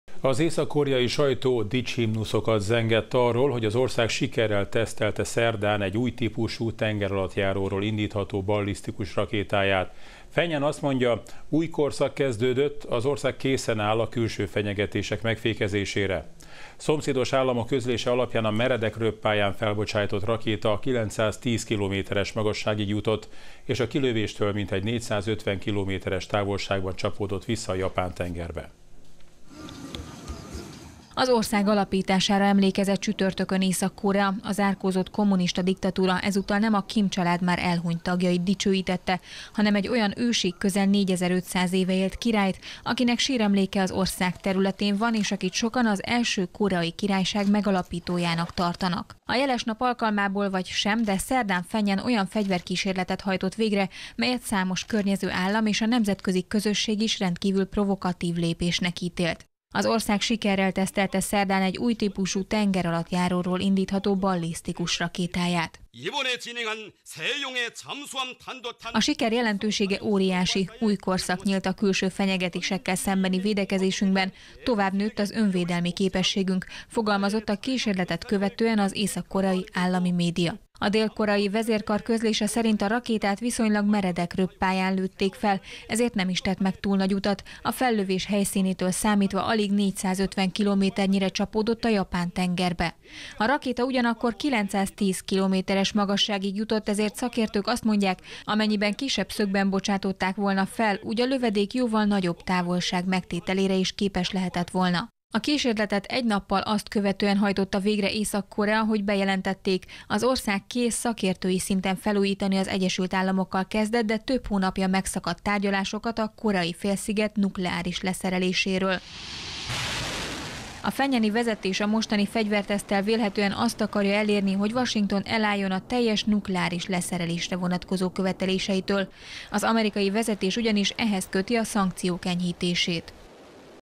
Tengeralattjáróról indítható rakétát tesztelt Észak-Korea
Az észak-koreai sajtó dicshimnuszokat zengett arról, hogy az ország sikerrel tesztelte szerdán egy új típusú, tengeralattjáróról indítható ballisztikus rakétáját.